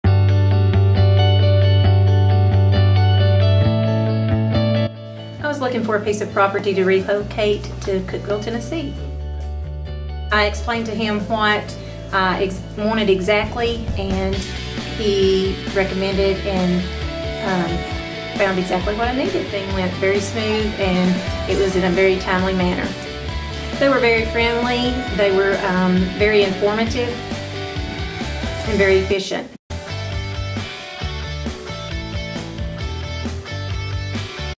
Ms. Lori
Ms. Lori found a new home in Cookeville!  Life happens, changes happen, ideas come to reality, and then we find ourselves in new homes.

Buy a home in The Upper Cumberland
Top REALTORs
Top selling team
Most 5-star reviews